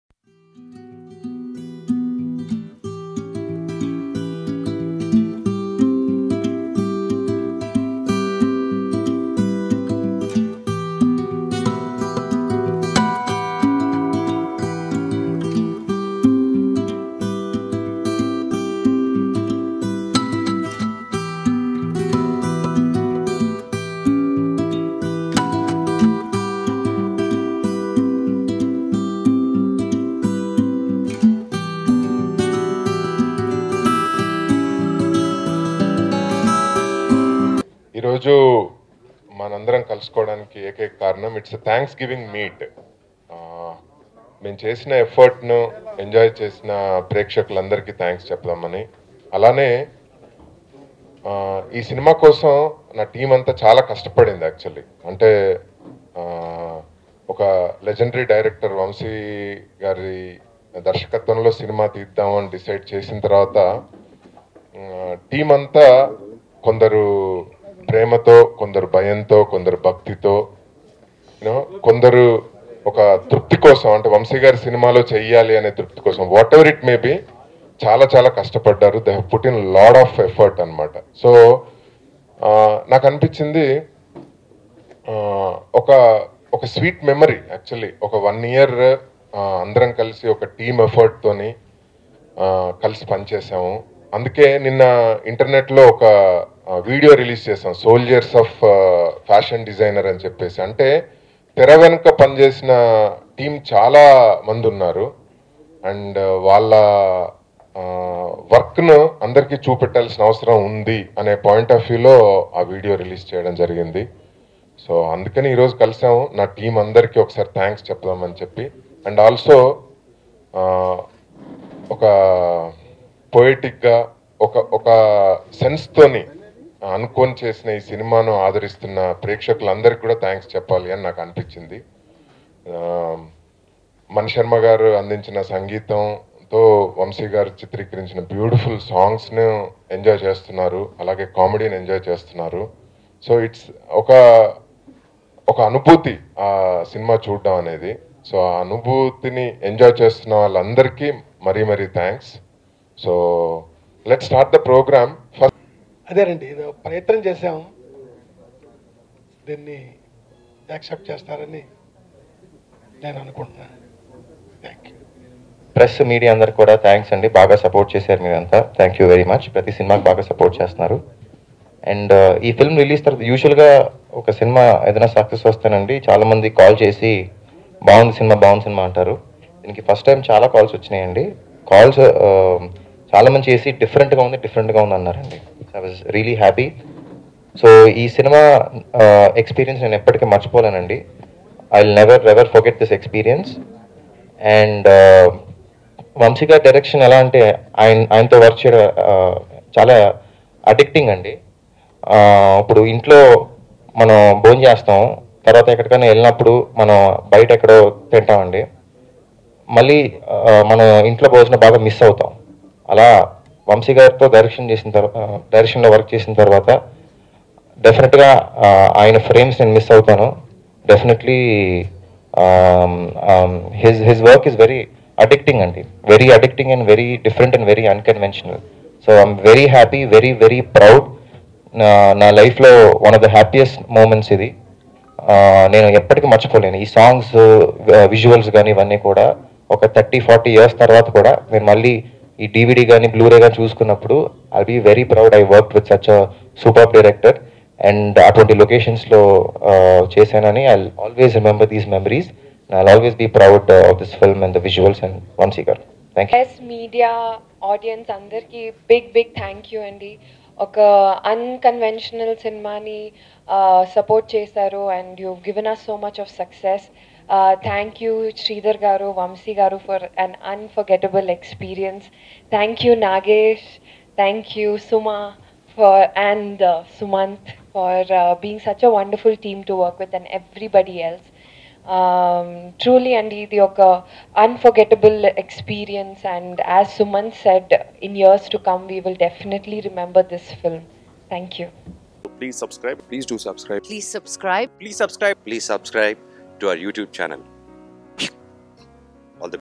Fashion Designer S/o Ladies Tailor Movie Success Meet | Sumanth | Anisha Ambrose | Manali | Manasa
tfpc tollywoodnews entertainment Watch Fashion Designer S/o Ladies Tailor Success Meet

Directed by Vamsy, Produced by Madhura Sreedhar Reddy, Music by Mani Sharma, Starring: Sumanth Ashwin, Anisha Ambrose, Manali Rathod & Manasa